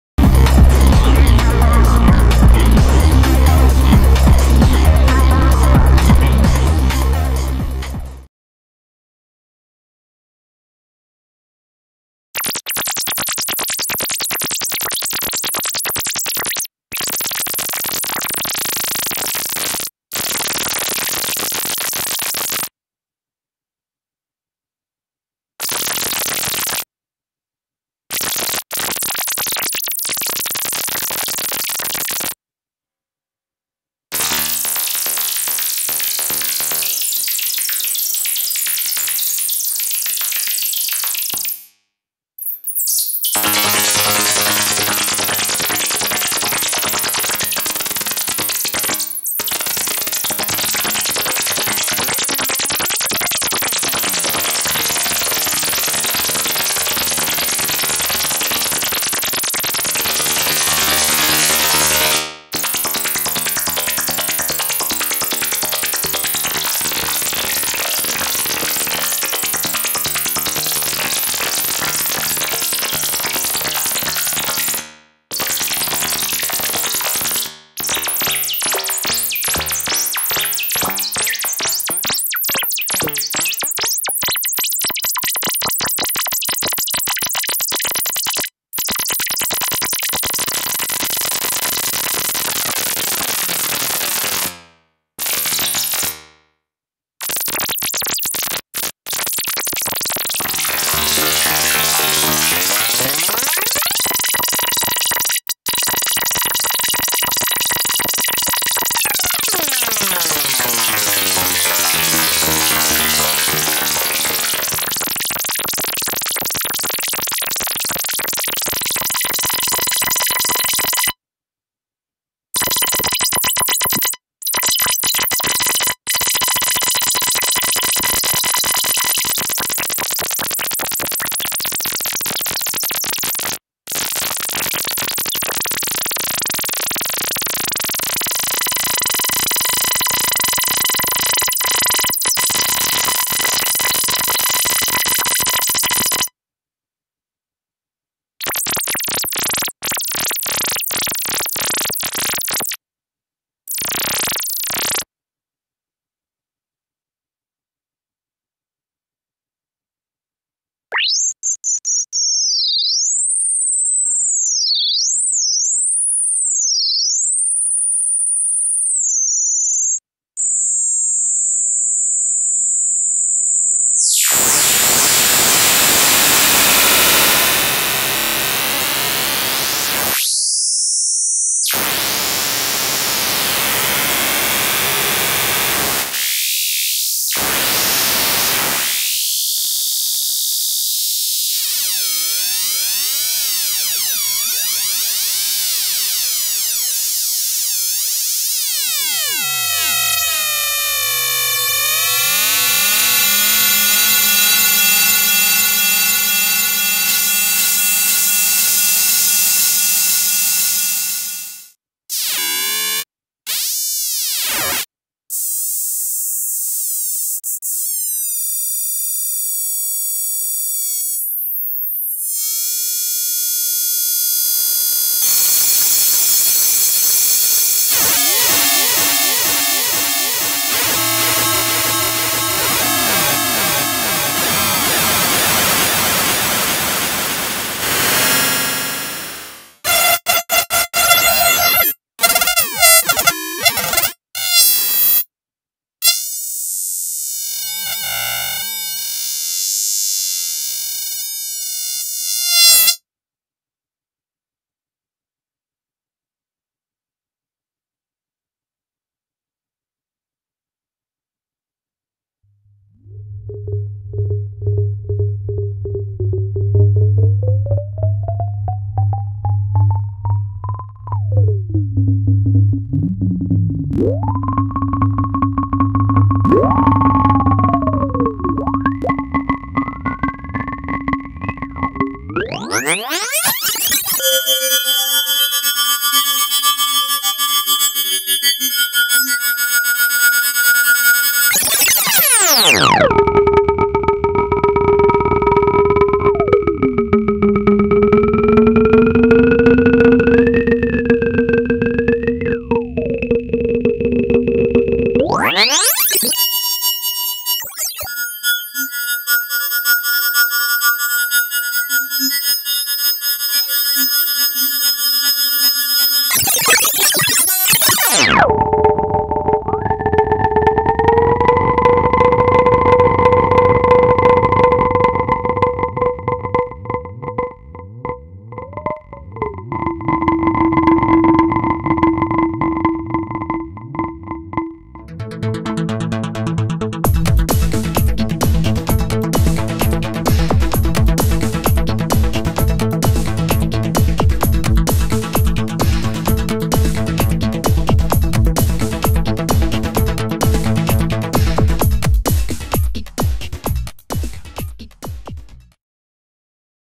Music Ipad App: Noisemusick
Music Ipad App first look at this amazing collection of 3 synthesizers Great for making strange sounds and odd noises. Get it for $0.99 from the itunes store.